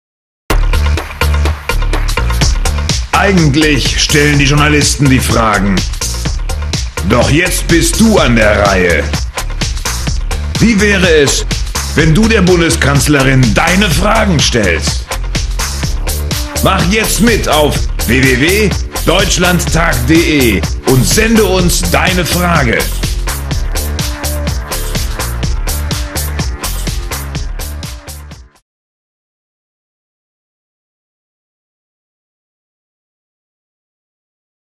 Deine Frage an den Deutschlandtag 2007
Nicht nur Delegierte, sondern jeder Interessierte hat jetzt online die Gelegenheit, Fragen an die Redner des Deutschlandtages zu stellen. Unter dem Motto „Deine Frage an den Deutschlandtag" kann unkompliziert und schnell ein Formular ausgefüllt werden.